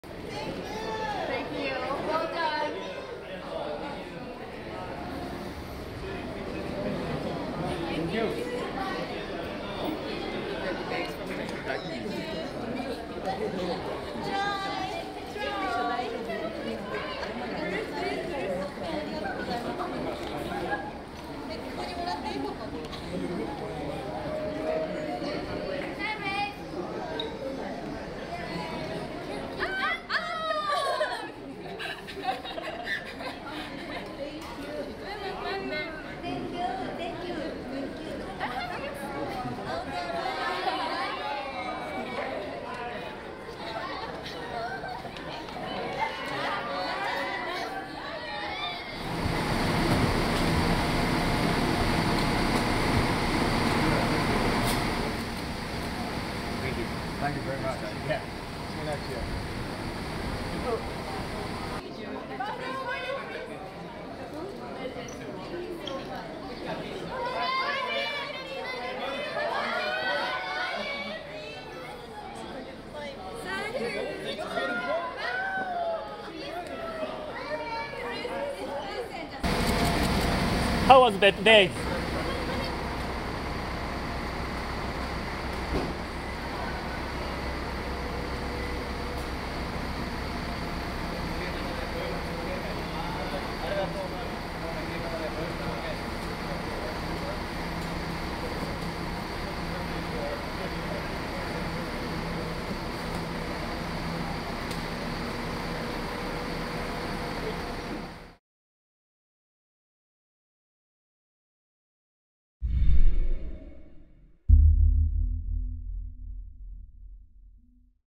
All Blacks farewell Beppu
The All Blacks say Arigato to the people of Oita as they depart Beppu for Tokyo to continue the rest of their Rugby World Cup campaign.